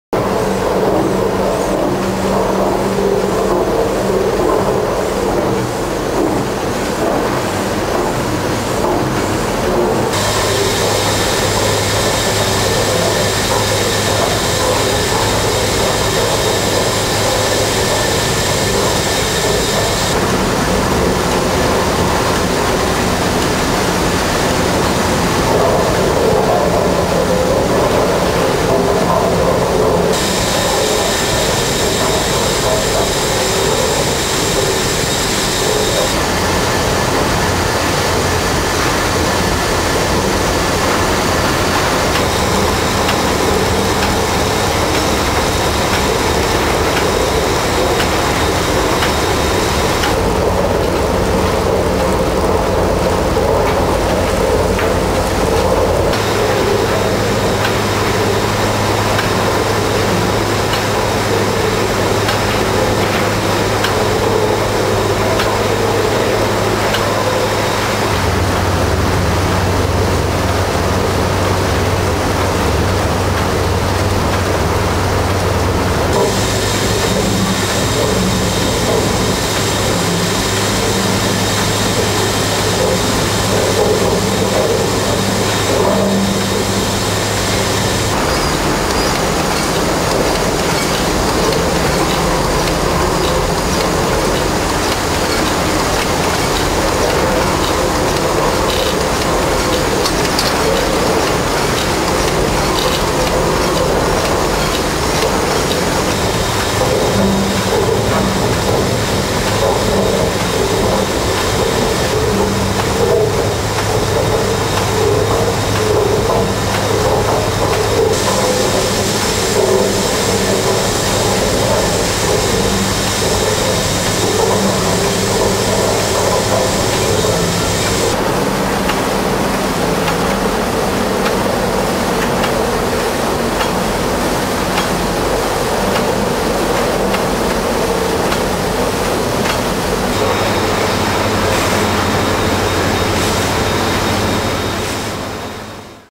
Gula Java 11, Tulangan Sugar Mill, Werkspoor Drop Valved Engine
Tulangan has two Werkspoor drop valved engines which are very special as they use the Meier Mattern hydraulic activation system. This is one of them.